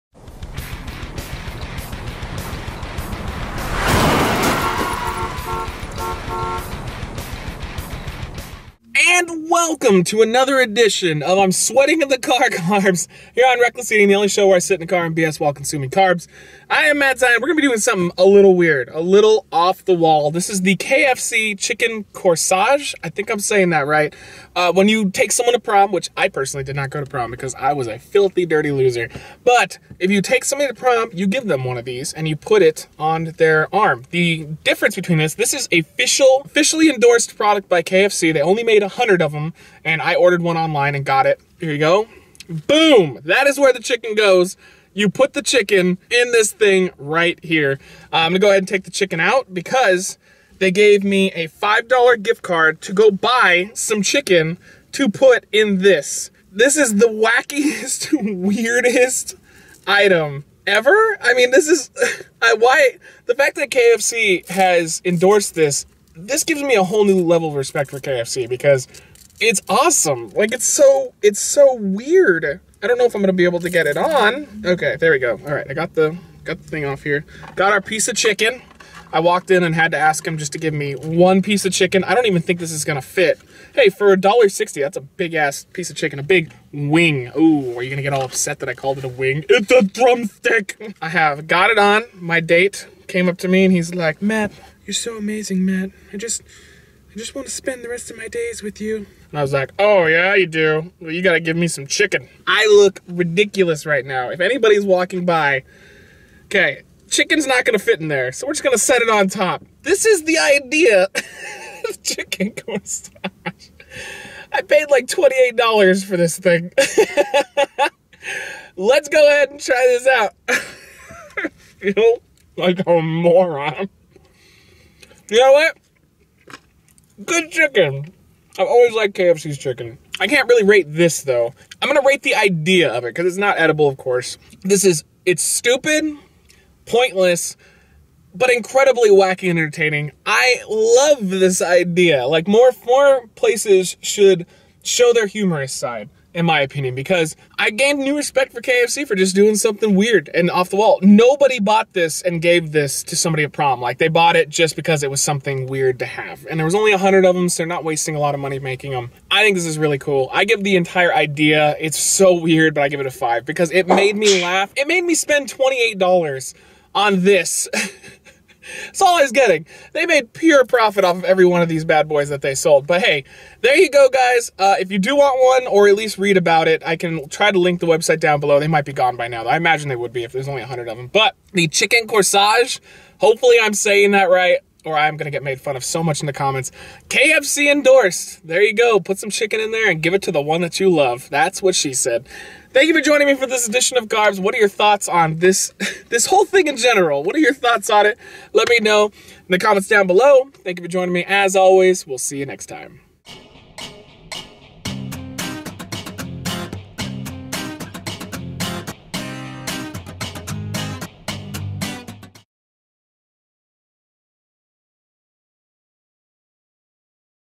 CarBS - KFC Chicken Corsage
Welcome to CarBS! On this show Matt Zion will be reviewing food/drinks in his car to get them out to the fans asap! Today Zion reviews KFC Chicken Corsage.